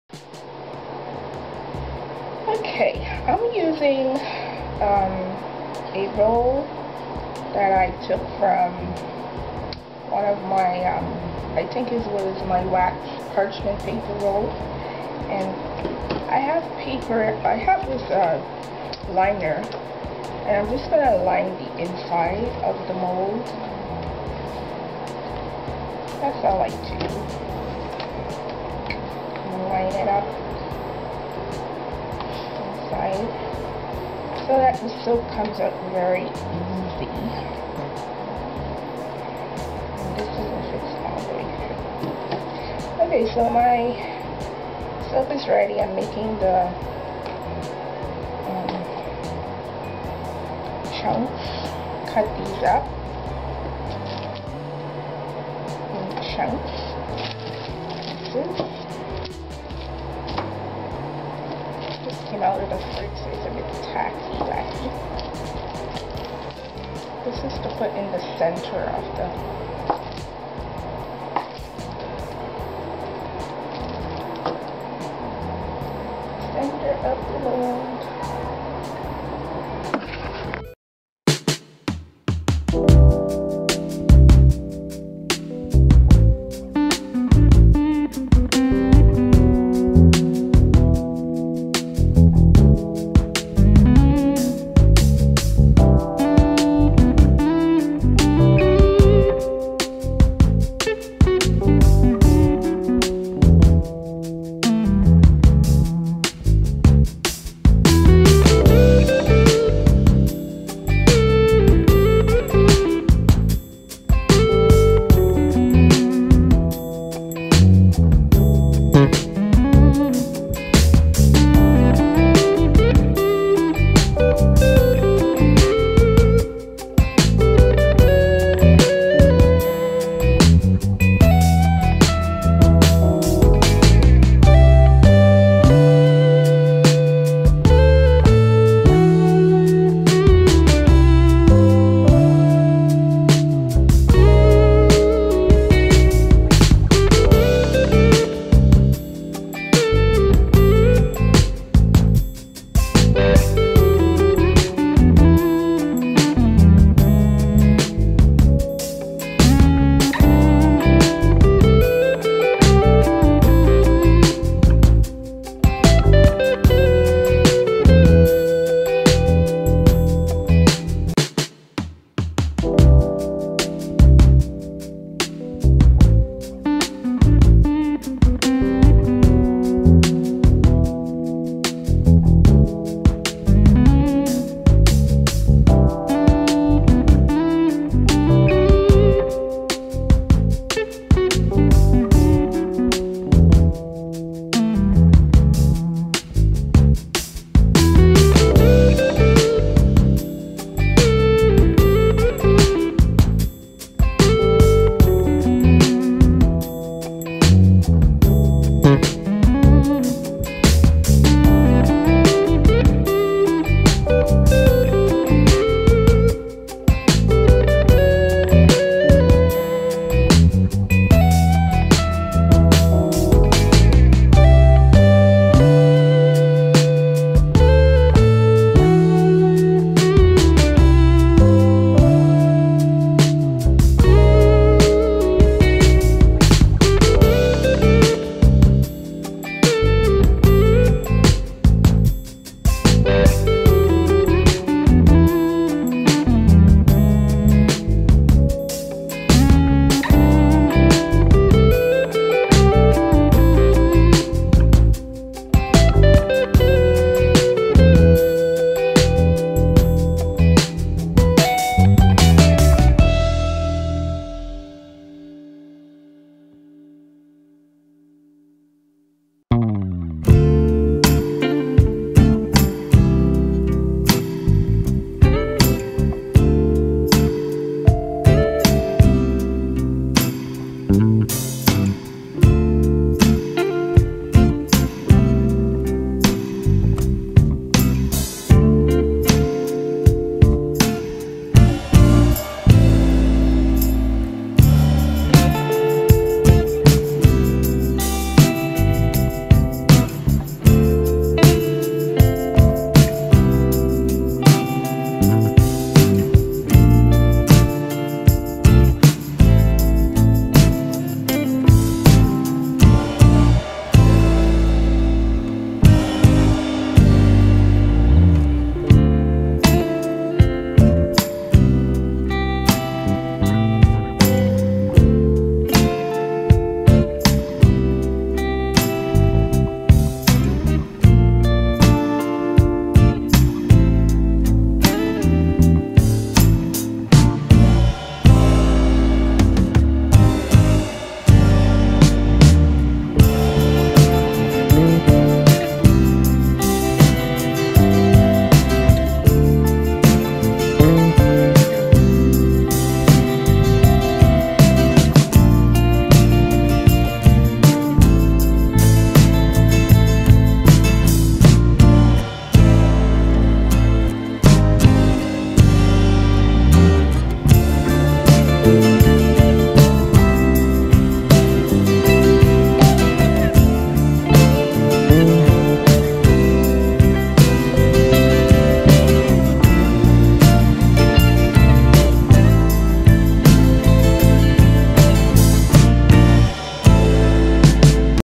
DIY How To Make Orange Slice Melt And Pour
Step by step making melt and pour soap into orange slices
Am using simple paper roll tubes with plastic liner easy to remove soap.